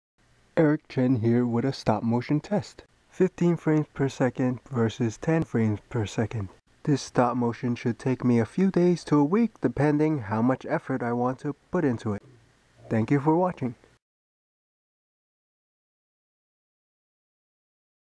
Gundam Stop Motion 15FPS vs 10 FPS Test GUNPLA STOP MOTION
A test on the difference between 15 frames per second and 10 frames per second.The first scene has more frames but each frame is shorter. The upcoming gundam stop motion will be GRUNTS: 0079 vs SEED